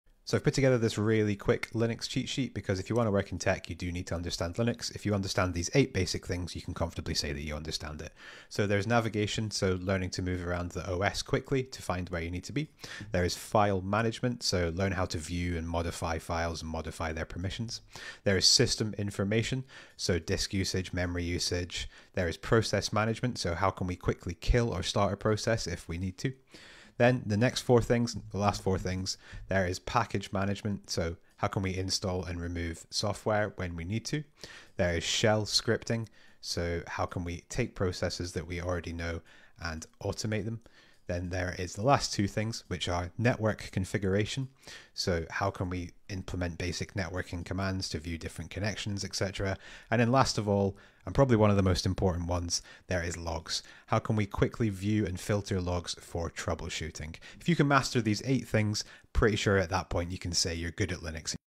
7 Steps to learn Linux
Learning Linux is pretty easy when you break it down